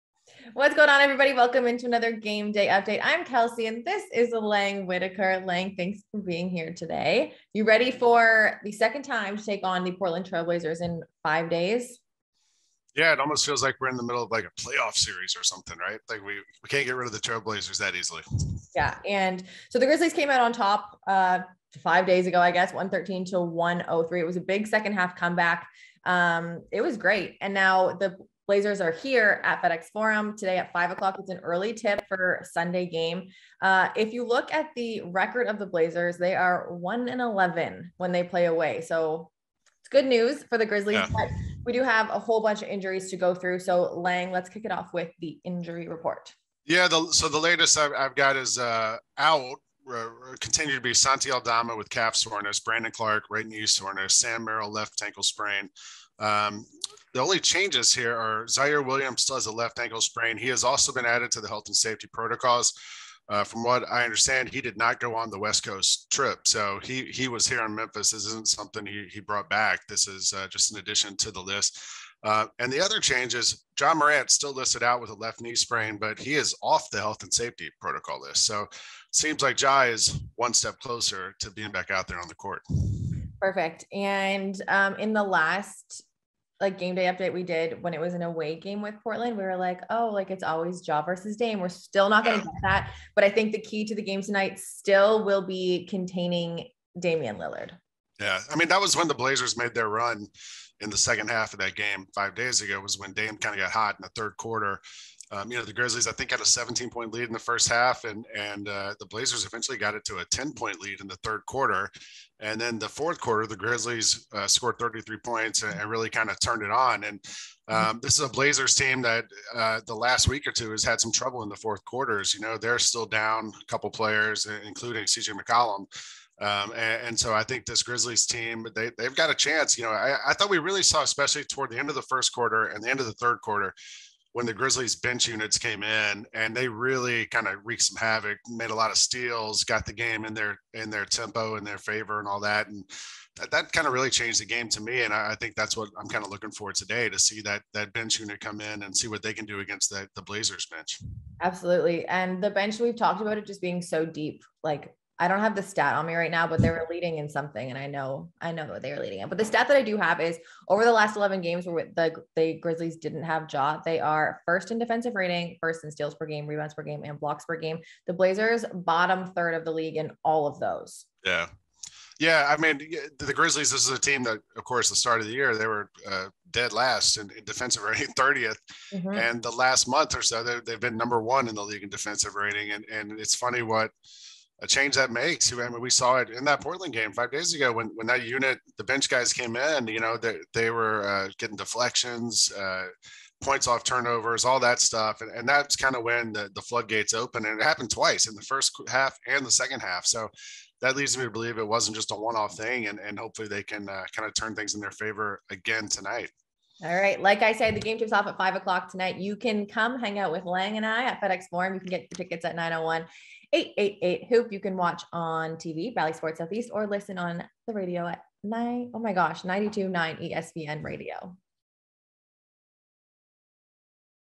Gameday Update: Grizzlies vs Trail Blazers 12.19.21
Grind City Media's Lang Whitaker and Kelcey Wright Johnson to discuss tonight's injury report, the Grizzlies bench unit, and the Grizzlies defensive success.